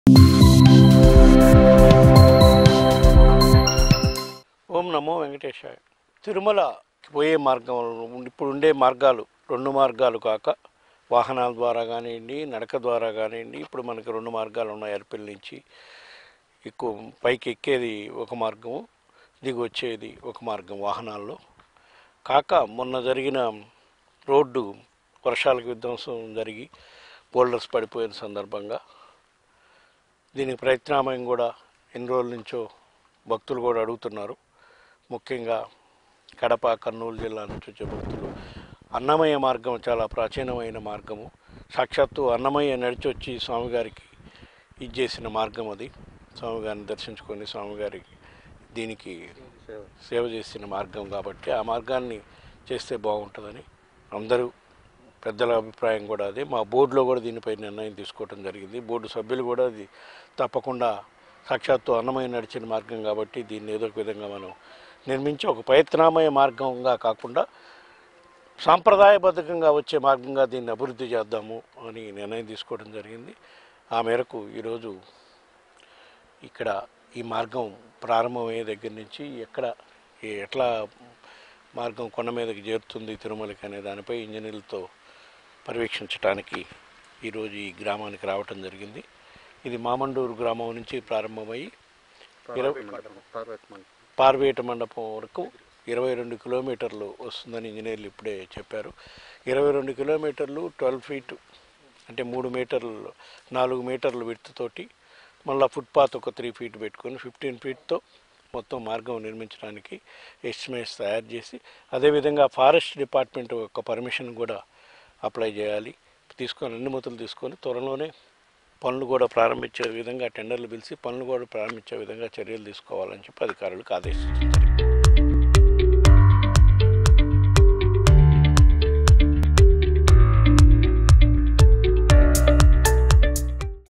TTD Chairman YV Subba Reddy Announces Third Road to Tirumala Tirupathi Devastanam | MEGA TV HD
Watch :
TTD Chairman YV Subba Reddy Announces Third Road to Tirumala Tirupathi Devastanam